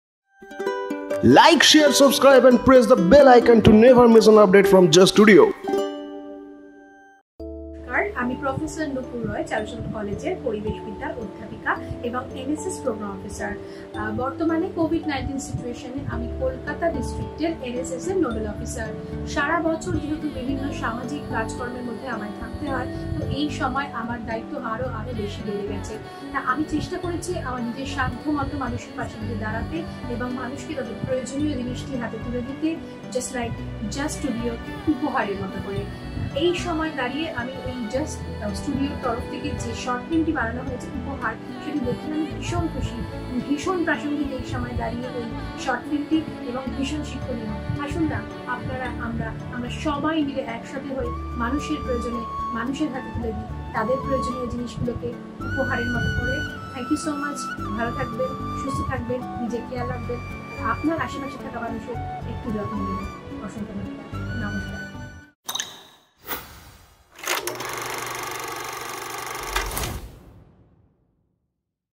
Nupur Roy | উপহার Reviews | The Gift | Just Studio Originals
Just Studio Originals এ সদ‍্য মুক্তি পেয়েছে স্বল্প দৈর্ঘ‍্যের ছবি 'উপহার'। ছবিটি দেখে প্রতিক্রিয়া জানালেন চারুচন্দ্র কলেজের অধ‍্যাপিকা নূপুর রায়।